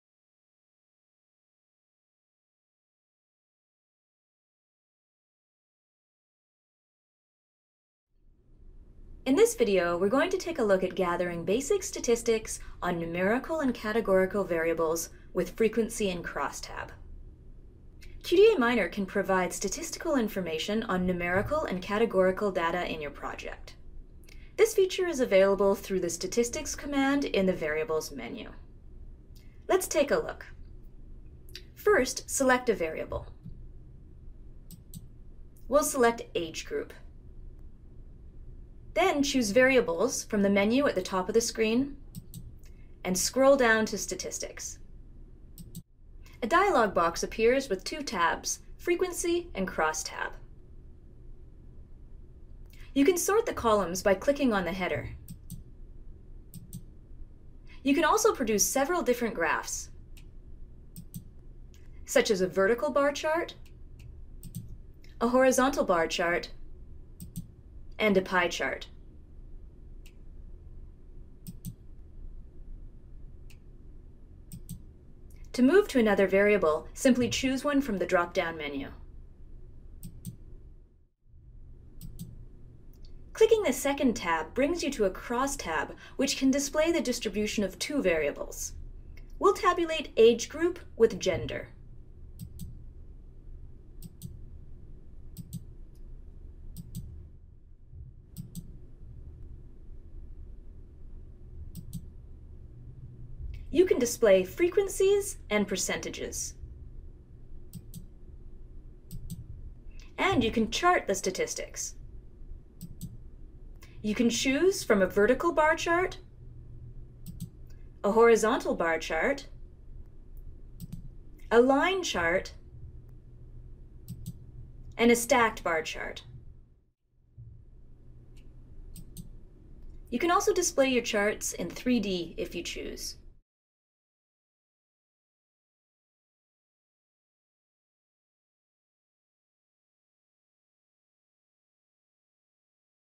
QDA Miner - Frequency Crosstab
How to quickly display frequencies of numerical and categorical variables.